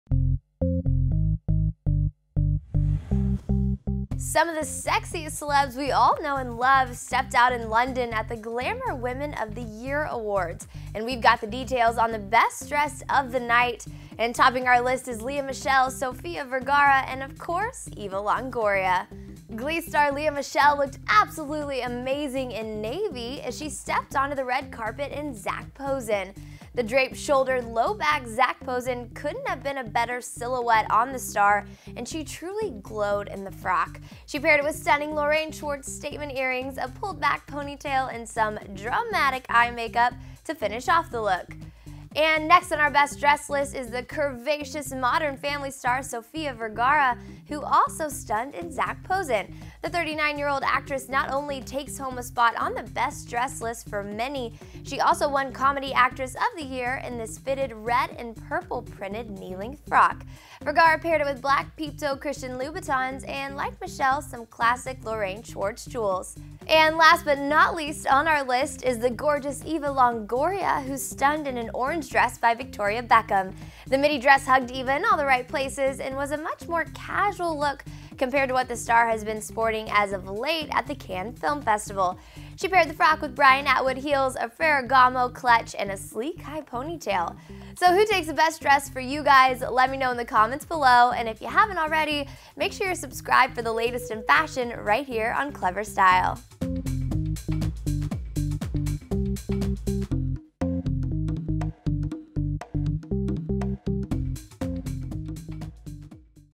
Best Dressed at the Glamour Women of the Year Awards 2012!
Some of the sexiest celebs we all know and love stepped out in London at the Glamour Women of the Year Awards and we've got the details on the best dressed of the night—topping our list, Lea Michele, Sofia Vergara and of course Eva Longoria.

Glee Star, Lea Michele looked absolutely amazing in Navy as she stepped onto the red carpet in Zac Posen.

The draped shouldered low back Zac Posen couldn't have been a better silhouette on the star and she truly glowed in the frock.

She paired it with stunning Lorraine Schwartz statement earrings, a pulled back ponytail and some dramatic eye makeup to finish off the look.

And next on our best dressed list is the curvaceous modern family star, Sofia Vergara who also stunned in Zac Posen!

The 39-year old actress not only takes home a spot on the best dressed list for many—she also won Comedy Actress of the Year in this fitted red and purple printed knee length frock.

Vergara paired it with black peep toe Christian Louboutins and like Michele, some classic Lorainne Schwartz jewels.

And last but not least on our list is the gorgeous Eva Longoria who stunned in an orange dress by Victoria Beckham.

The midi dress hugged Eva in all the right places and was a much more casual look compared to what the star has been sporting as of late at Canne Film Festival.

She paired the frock with Brian Atwood Heels, a Ferragama clutch and a sleek high pony tail!

I'm Brianna Baxter for Daily Style.